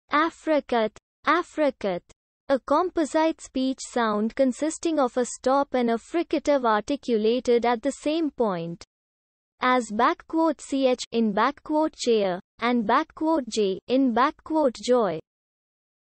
'Affricate' Meaning and Pronunciation
Word: Affricate
Meaning: a composite speech sound consisting of a stop and a fricative articulated at the same point (as `ch' in `chair' and `j' in `joy')